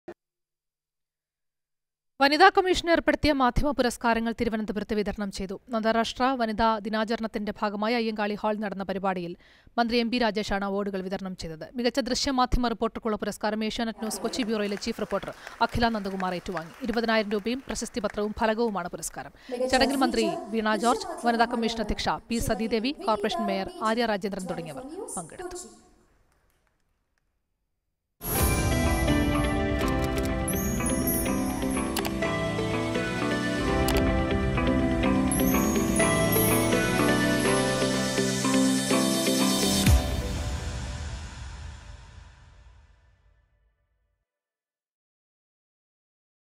വനിത കമ്മീഷന്‍ മാധ്യമപുരസ്കാരം വിതരണം ചെയ്തു | Women's Commission Media Awards
വനിത കമ്മീഷന്‍ മാധ്യമപുരസ്കാരം വിതരണം ചെയ്തു, മികച്ച ദൃശ്യമാധ്യമ റിപ്പോർട്ടർക്കുള്ള പുരസ്കാരം ഏഷ്യാനെറ്റ് ന്യൂസ് ചീഫ് റിപ്പോർട്ടർ അഖില നന്ദകുമാർ ഏറ്റുവാങ്ങി